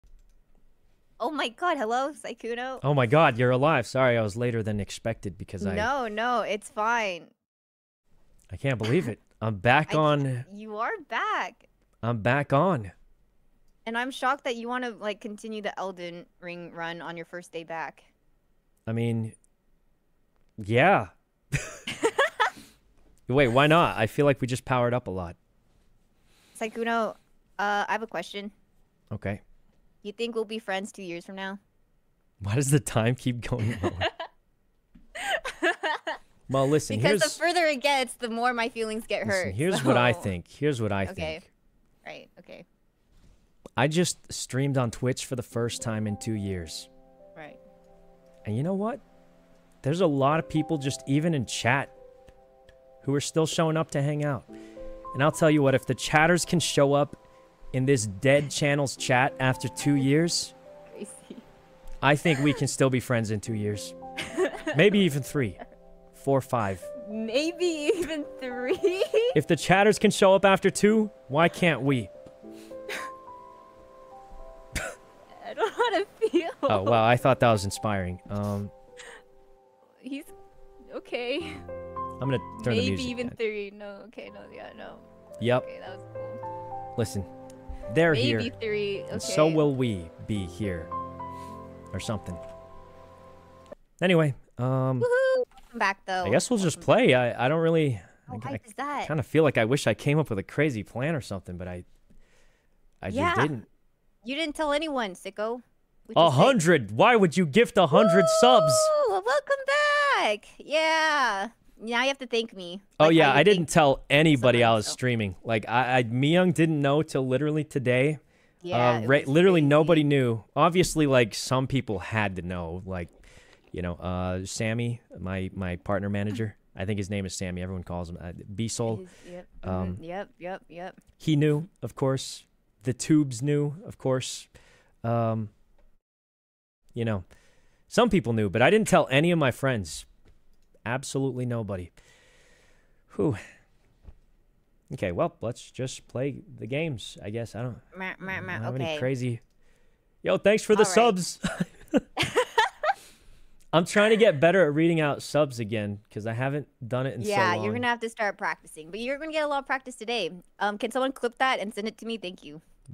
Sykkuno made a surprise return to Twitch after two years
Follow Sykkuno!